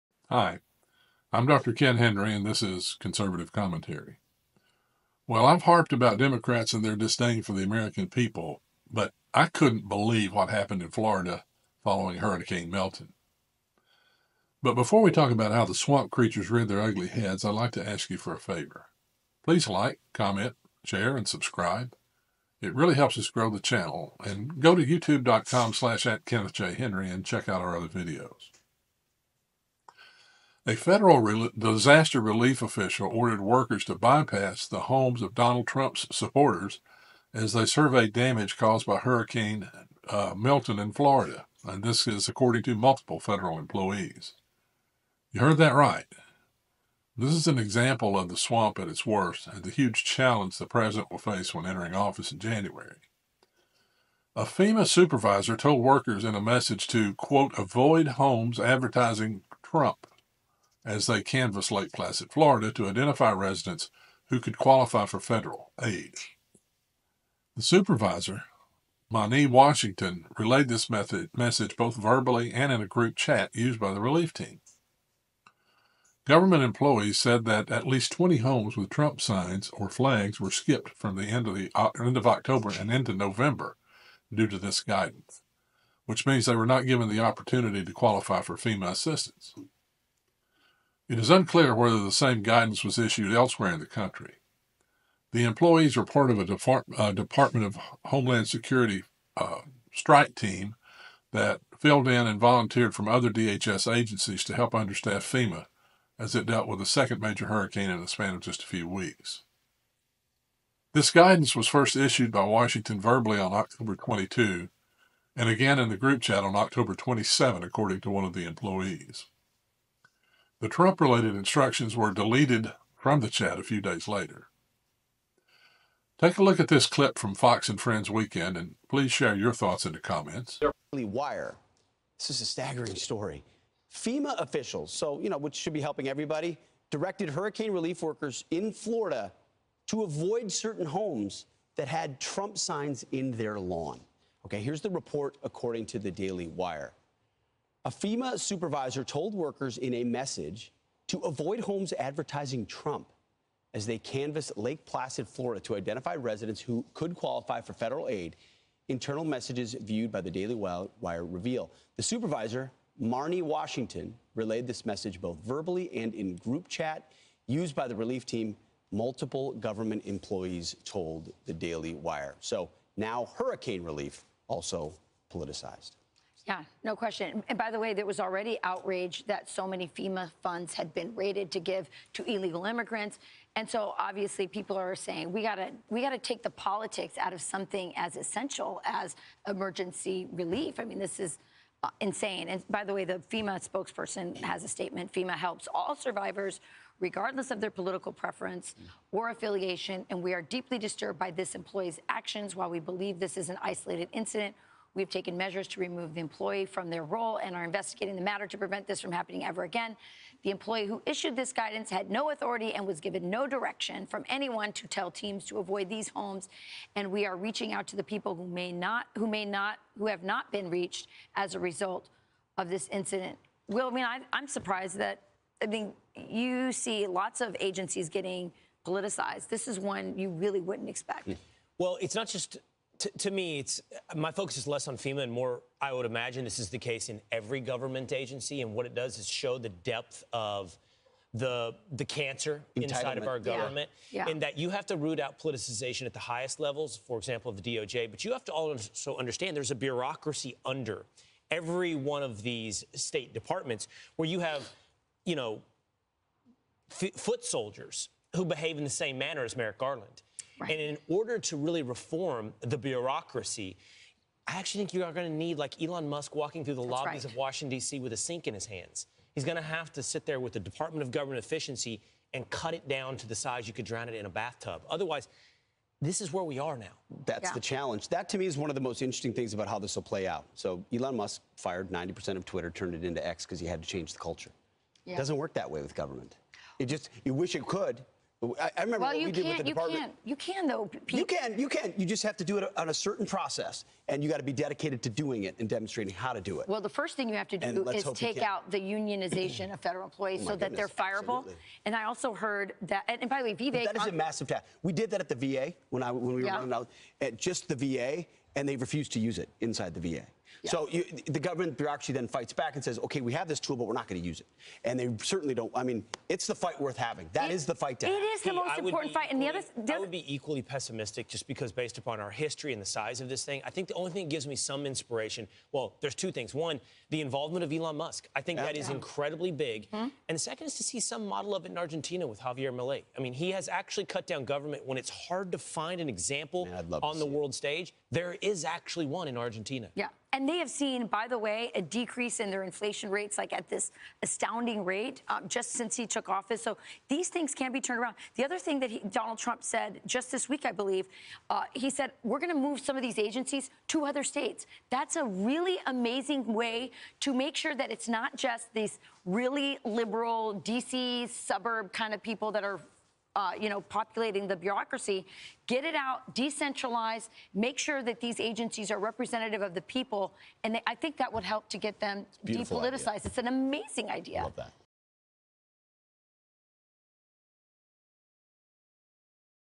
FEMA Refuses Services to Trump Supporters: The Swamp is Everywhere
A FEMA supervisor instructed workers canvassing after Hurricane Milton to pass up houses with Trump signs or banners.  This show how deep the swamp really is.